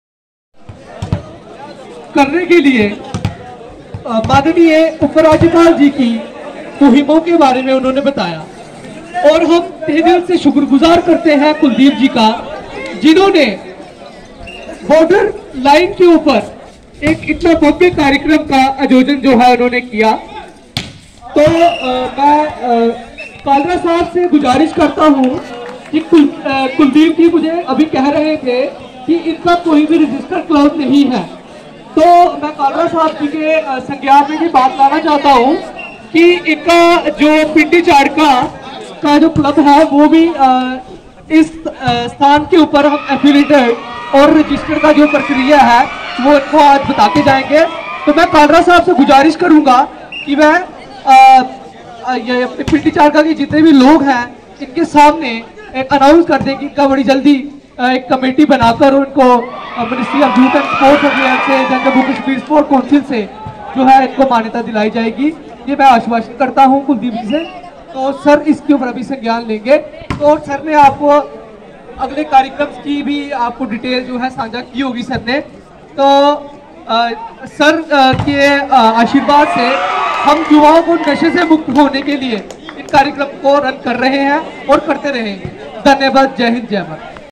Volleyball tournament organized at Panchayat Pindi Charaka Arnia
JAMMU, Apr 3: A two day Volleyball tournament, organized by Panchayat Committee- Arnia along the International Border with the support of J&K Sports Council, concluded at Pindi Charaka, near here today.
Eighteen teams from the various villages along the International Border participated in the tournament, wherein Ricky Volleyball Club emerged as winner in a keenly fought final by defeating KP Volleyball Club.
Ranjeet Kalra, Member J&K Sports Council gave away the trophies to the winners and also distributed medals among the players.
Neelam Singh Sarpanch Pindi Charaka Panchayat along with Naib Sarpanch Kuldeep Raj and prominent people of the area expressed gratitude to Lieutenant Governor Manoj Sinha (President of the J&K Sports Council) for his ‘Har Din Khel Sab Ke Liye Khel’ initiative and Secretary Sports Council, Nuzzat Gul for helping the organizing committee for holding the tournament in Arnia sector of the international border.
Speaking on the occasion, Ranjeet Kalra assured the Panchayat office bearers and sports clubs for Council’s support in their future sporting events. He also informed that various initiatives are being taken by the Sports Council ‘My Youth My Pride’ program to engage the youth.
The tournament was organized in the loving memory of late Rakesh Singh (Sunny)- a popular Volleyball player of the area.
Ram Singh, father of late Rakesh Singh also expressed gratitude to the Sports Council and organizing committee for organizing the tournament in the memory of his son who was a national level player.
“Such types of tournaments not only promote talented sports persons but also help to keep the youth away from the menace of drugs, ” Ram Singh said.
Prominent persons including Kamal Kumar, Tarsem Lal Sangra, Prabhat Singh, Kuldeep Singh (JKP) Ghar Singh, Roop Lal, Sat Paul, Sachan Sharma, Sushil Singh Charak, Chairman NDA, Bunny Sharma, Karan Singh Bantu and Vicky Kumar were present during the event.